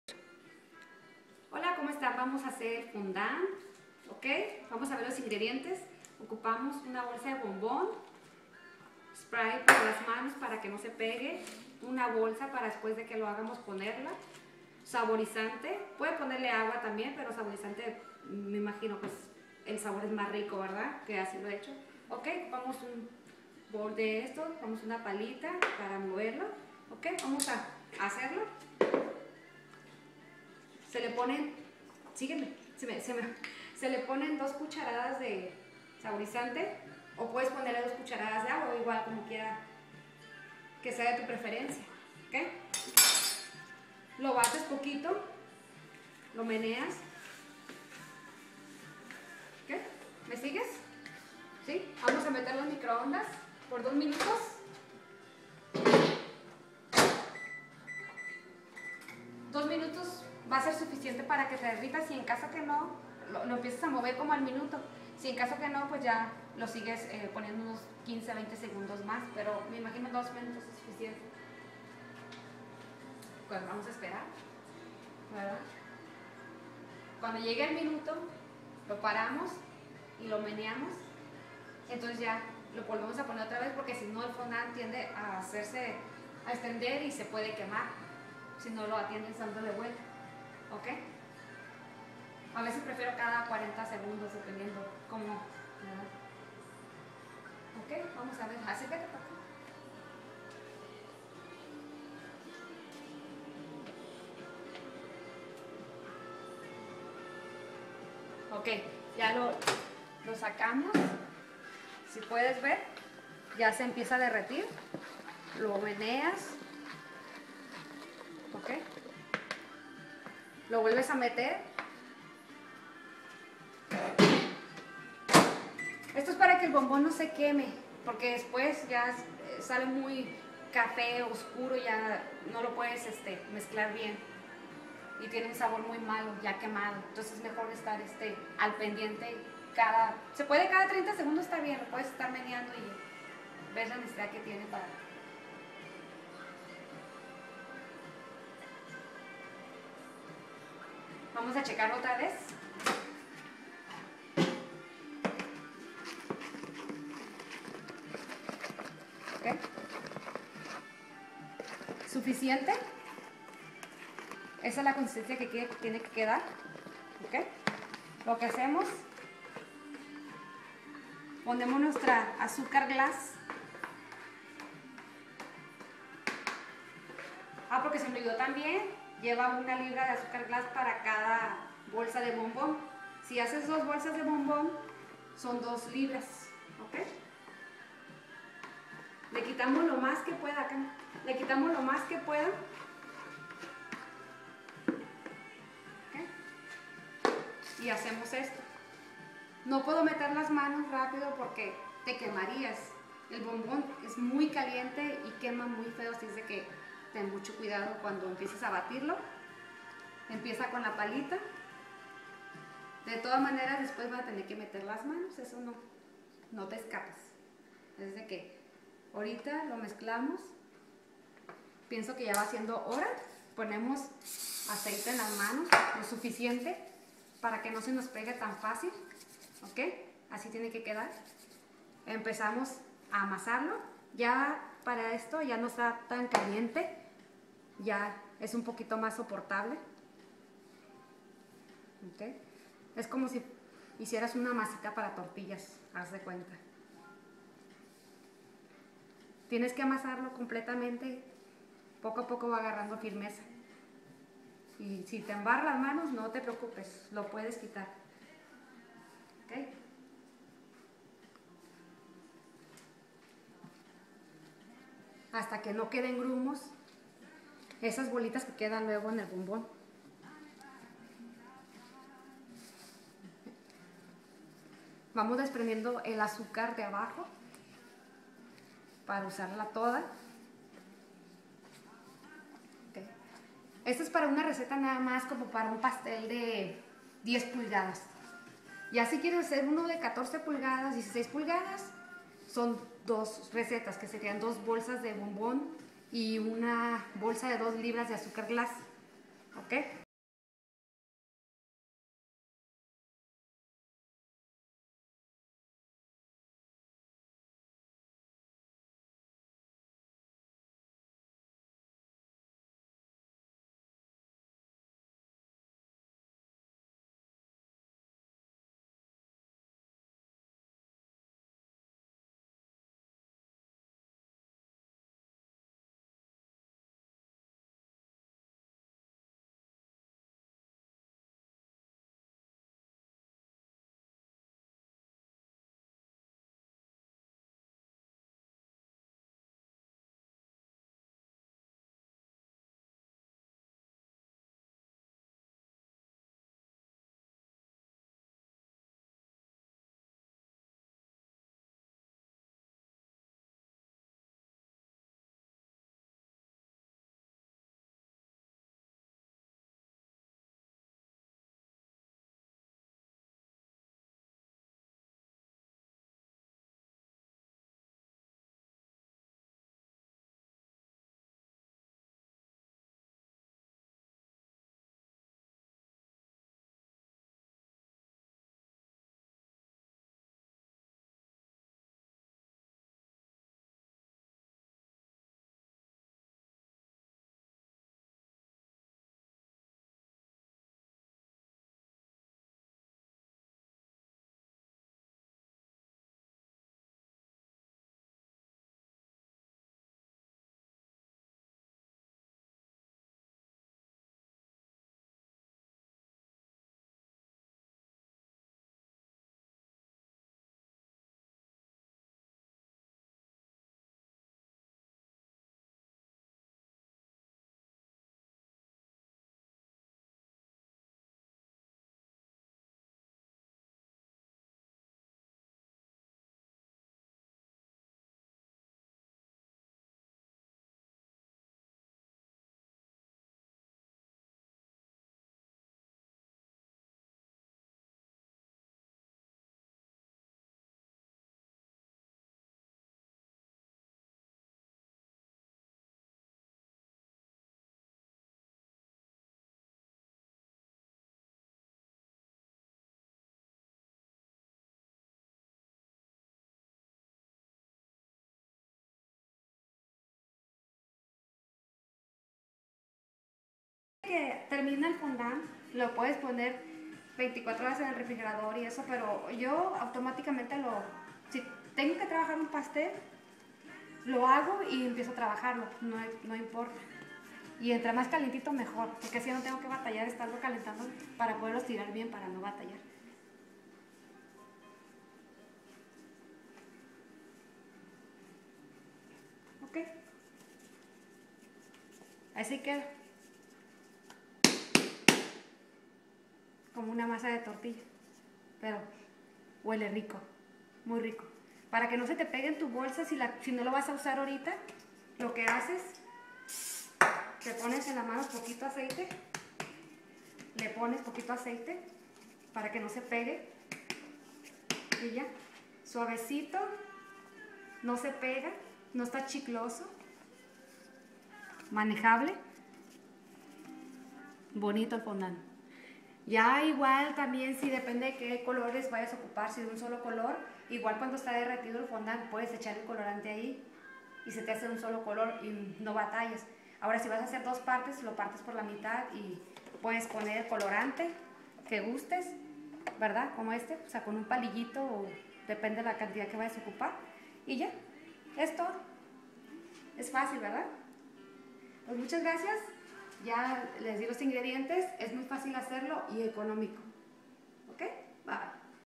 Como hacer fondant de bombom
aqui estan los ingredientes que se necesitan para realizar este fondant, 1 libra de azucar glass o lo mismo que casi medio kilo de azucar glass 1 bolsa de bombom color blanco de 10 onzas o lo mismo que 284 gramos 2 tablespoon de agua o saborizante color claro aceite o manteca vegetal para tus manos para que no se te pegue.